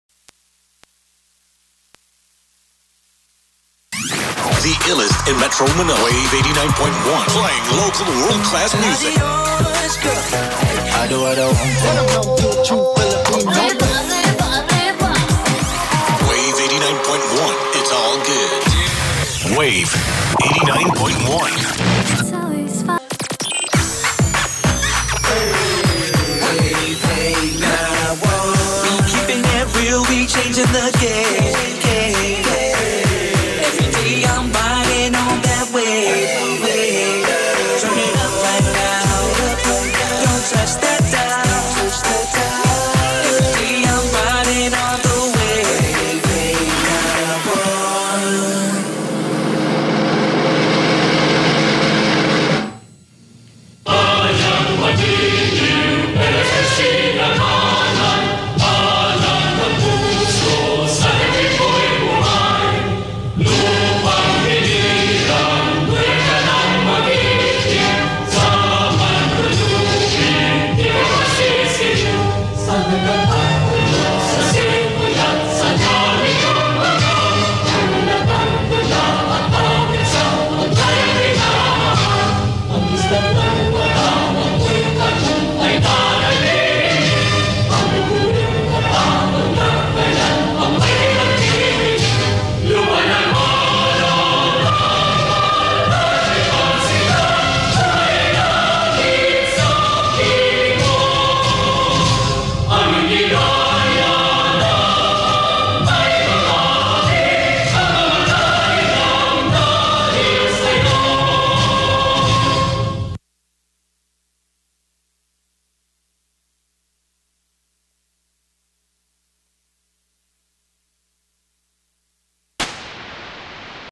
Wave 89.1 Sign on and Sign off (December 2021)
Recorded from December 5, 2021 at 6am and December 6, 2021 at 8pm

This station operates daily from 6am to 8pm (same with 103.5 K-Lite) due to effects of COVID 19 Pandemic.